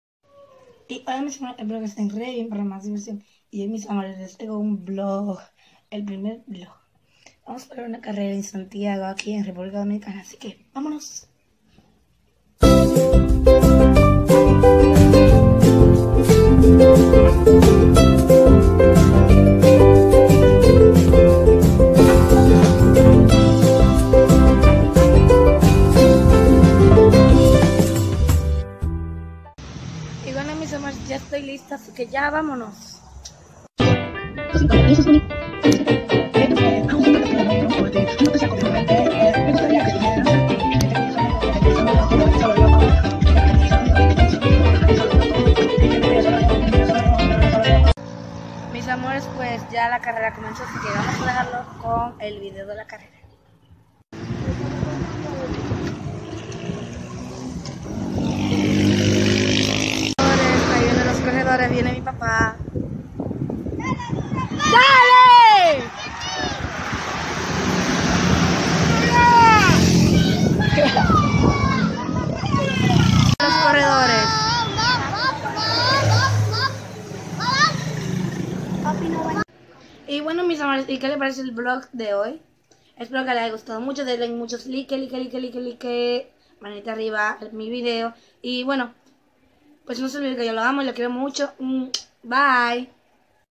#1 Vlog carrera en santiago
Mis amores es mi primera vez blogueando y no se mucho espero que les haya gustado mucho mis amores los quiero mucho ♡♡♡♡♡♡